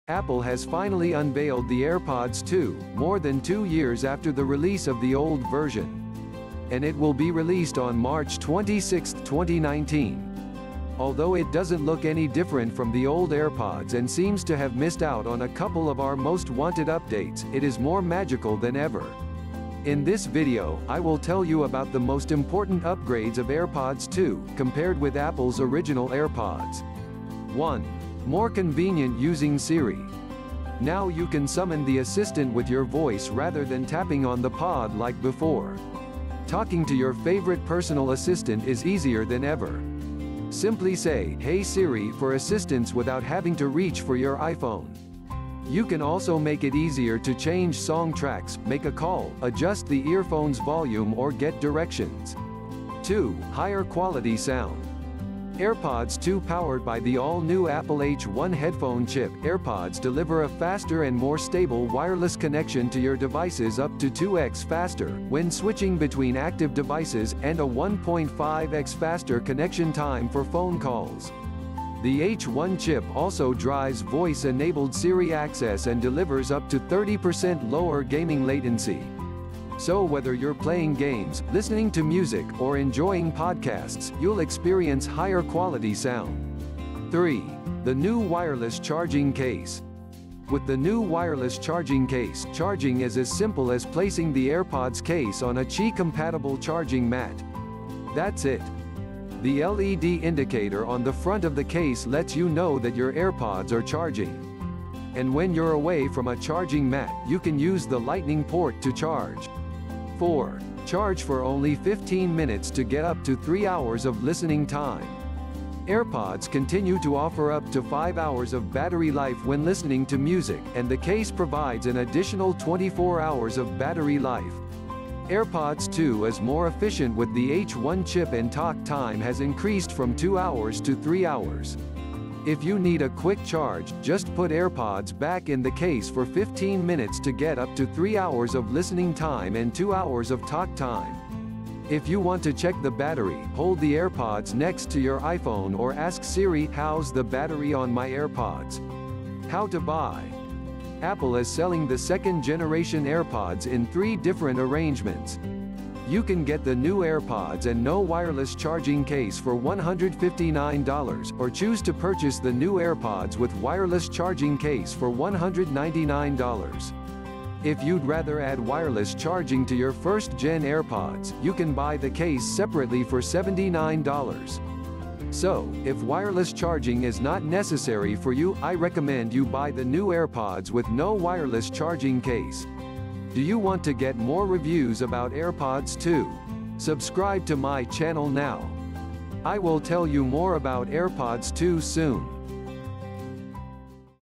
AirPods 2 vs AirPods 1 | The Most Important Upgrades of 2019 AirPods 2
Apple AirPods with Charging Case (Latest Model)
Apple has finally unveiled the AirPods 2, more than two years after the release of the old version.
And it will be released on March 26, 2019.

Although it doesn't look any different from the old AirPods and seems to have missed out on a couple of our most-wanted updates, it is more magical than ever.

In this video, I will tell you about the most important upgrades of AirPods 2, compared with Apple's original AirPods.